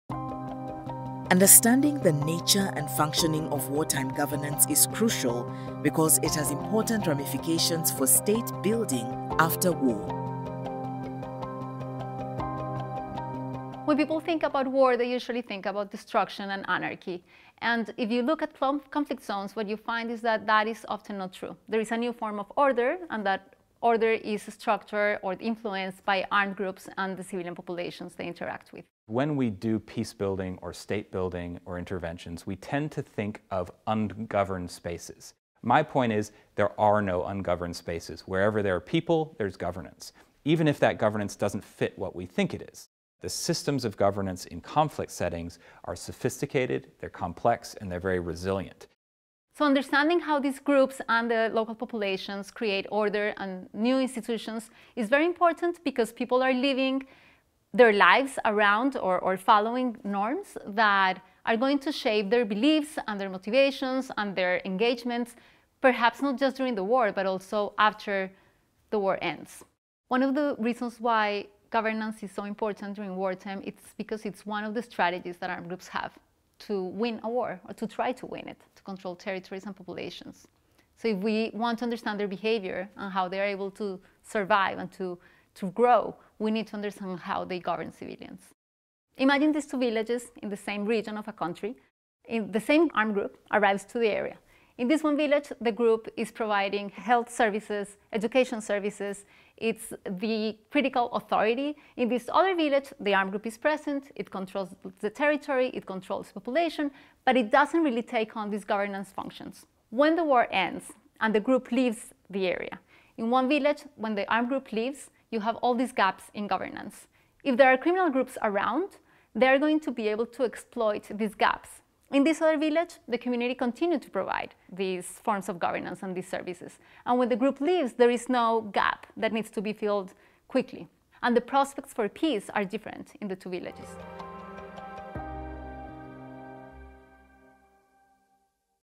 The puzzle of peace #2: Legacies of wartime governance
Understanding the nature and functioning of wartime governance is crucial because it has important ramifications for state building after war.

“When people think about war, they usually think about destruction and anarchy. And if you look at conflict zones, what you find is that that is often not true. There is a new form of order, and that order is the structure or the influence by armed groups on the civilian populations they interact with.” - Ana Arjona

This video is the second in our “Puzzle of peace” series, which introduces some of the key issues in peace and conflict research, featuring various experts involved in our research project.

Find out more: wider.unu.edu/conflict